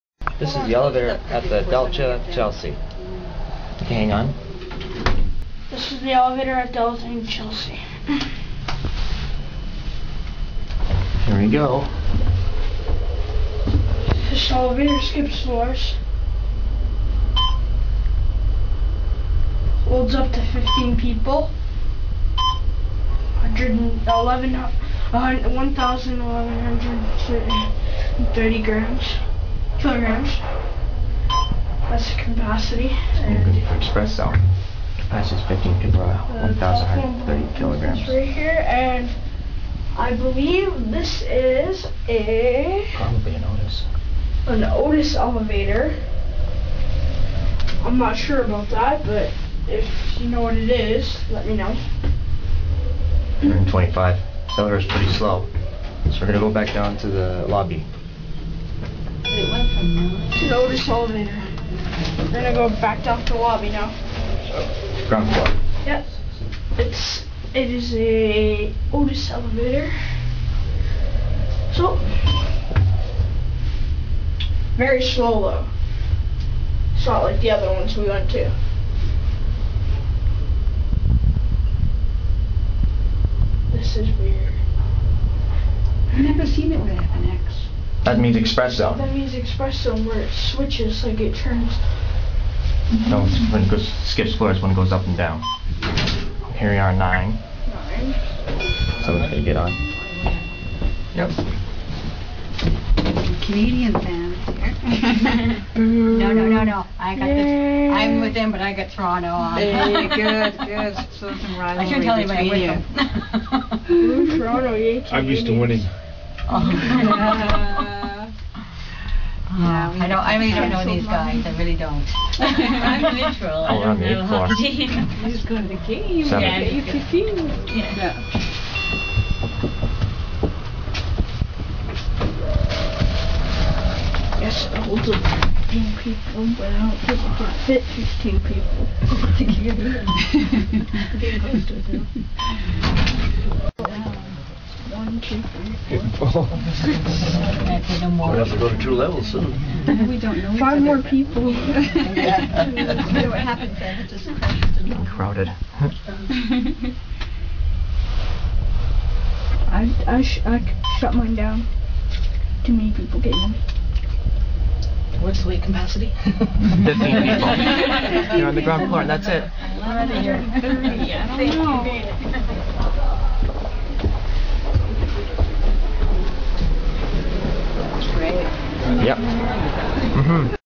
Westinghouse Traction Elevator @ The Delta Chelsea (G-3, 5-9, 25) Downtown Toronto ON
Riding the very nice elevator at the Delta Chelsea serving floors G-3, 5-9, 25. This is my first Westinghouse!

Tech. Specs:

General: Westinghouse/Geared Traction

Capacity: 15 people/1130kg/2500lbs

Speed & Dist: 25 floors (G-3, 5-9, 25)/255ft/350FPM

Power: 10HP

Door Safety: Full Sensor

Year Installed: 1975

Modernized by: Schindler